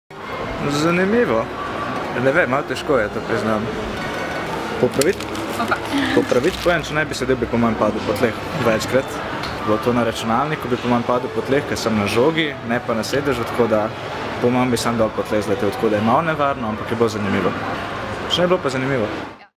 Vtisi virtualne igre VR Ptica
ts.si/vr-ptica

VR Ptica je virtualna igra Telekoma Slovenije, v kateri igralec s pomočjo navidezne resničnosti poleti kot ptica.